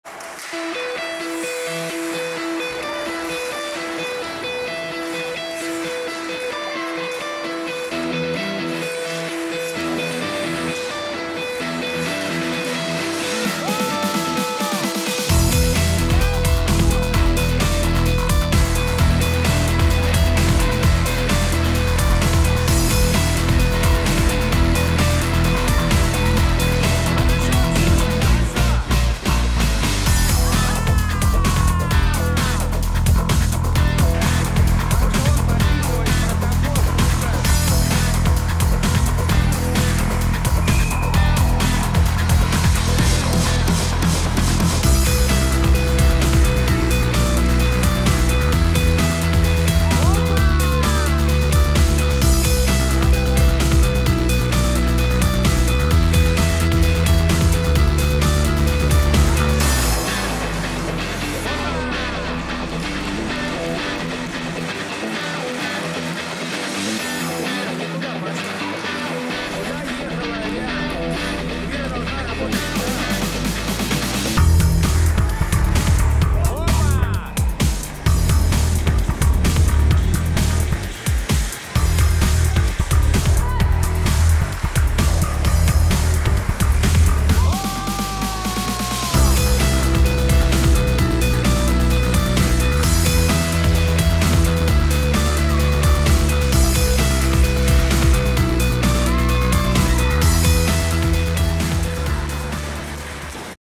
2016-12-04. Все голы Искры
Первенство Республики Татарстан по хоккею среди девушек.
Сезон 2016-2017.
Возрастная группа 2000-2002